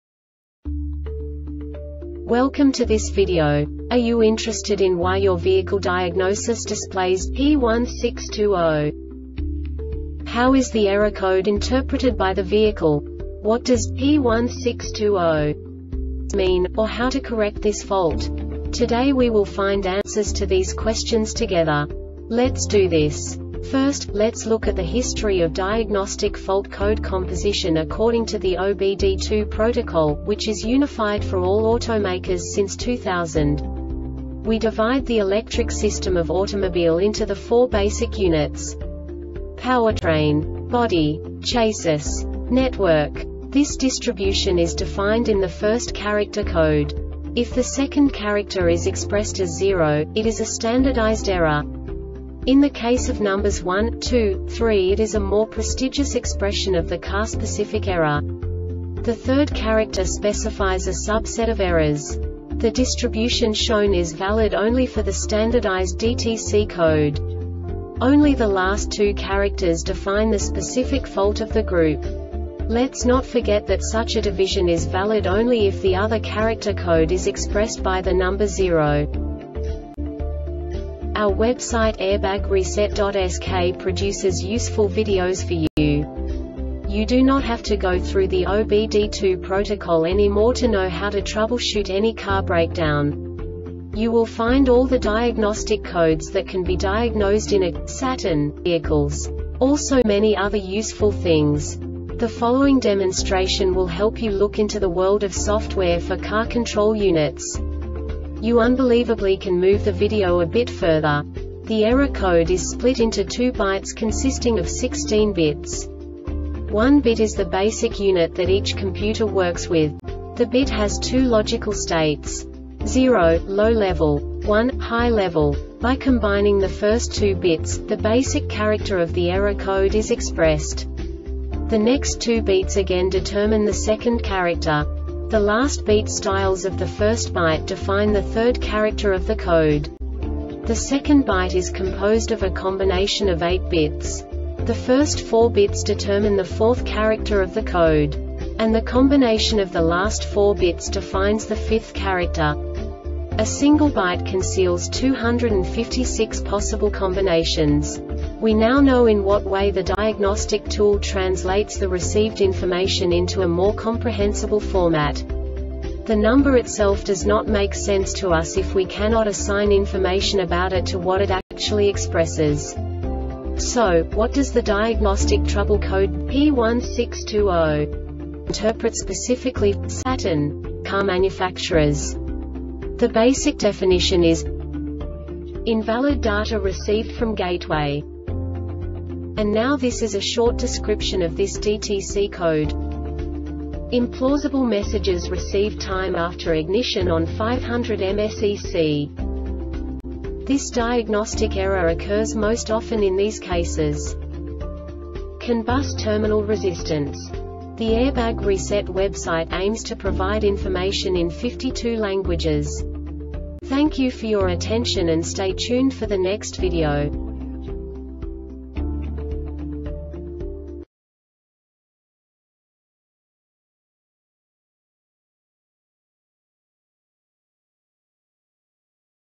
DTC Saturn P1620 Short Explanation
Contents:
0:21 Basic DTC analysis according to OBD2 protocol standard.
1:48 Insight into programming
2:58 A diagnostically understandable description of the Diagnostic Trouble Code
3:05 Basic error definition

Make:
Saturn
Code:
P1620
Definition:
Invalid Data Received From Gateway
Description:
Implausible messages received.Time after ignition on 500 mSec.
Cause:
 CAN Bus terminal resistance